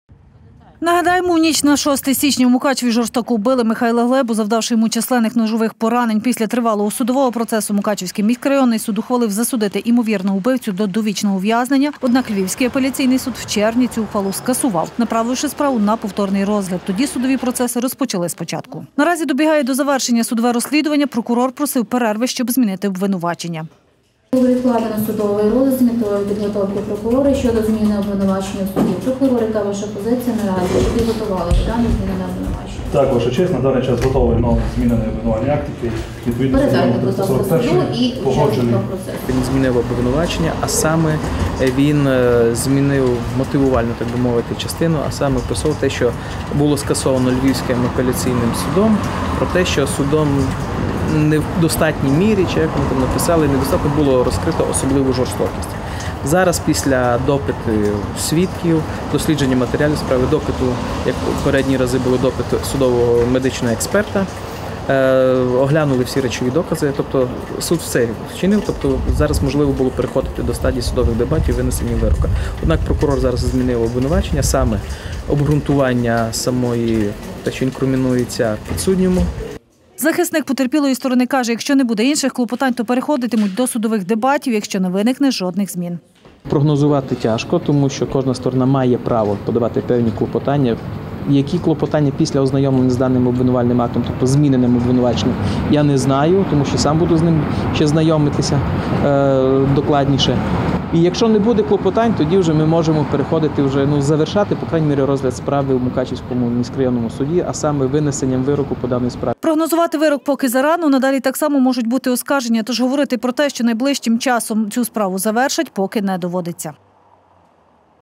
Cуд триває
У резонансній справі вбивства Михайла Глеби у Мукачеві і надалі тривають судові засідання, адвокат потерпілої сторони каже – наразі слухання майже на завершальному етапі. Чим закінчиться ця резонансна справа, стверджувати не беруться, адже попередню ухвалу Мукачівського суду, згідно з якою імовірний злорчинець був засуджений, Львівський апеляційний суд скасував.